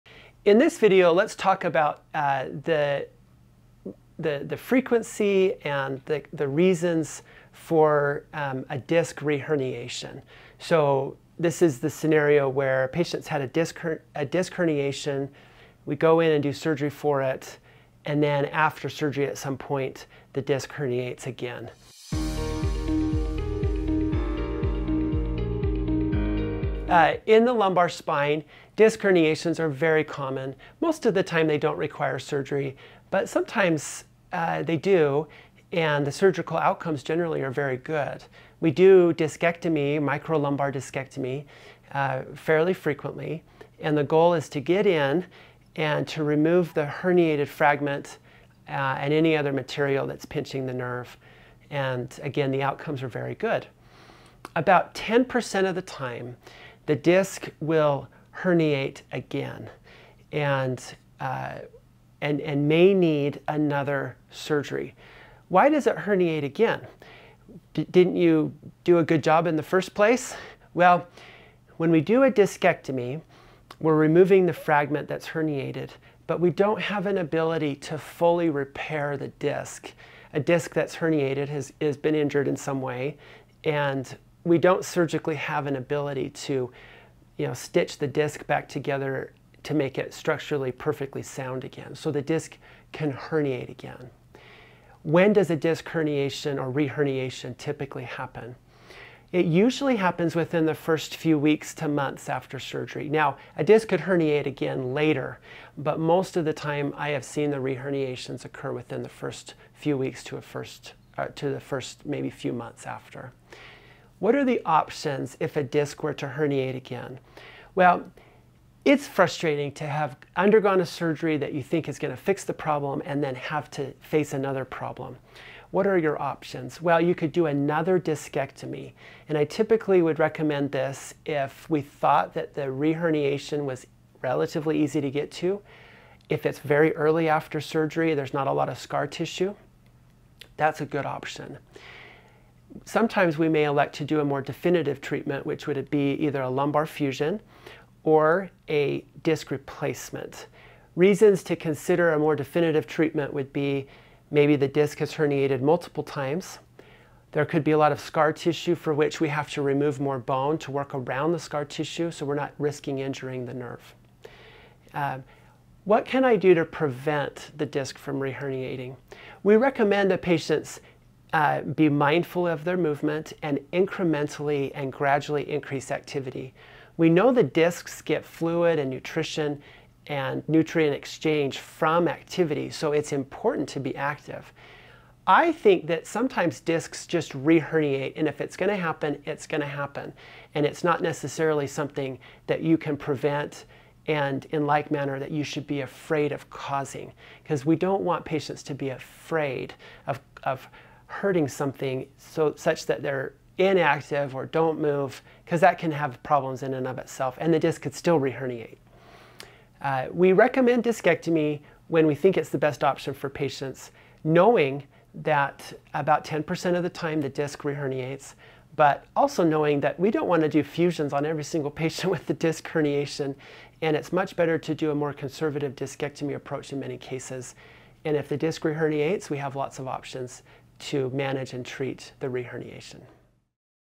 Disc reherniation // why it happens and what to do
Follow our Instagram @summitbrainandspine

0:00 intro
0:26 about herniations and surgery
1:05 why it reherniates
1:35 when does reherniation happen
1:54 options after reherniation
2:44 preventing reherniation
3:30 conclusion/summary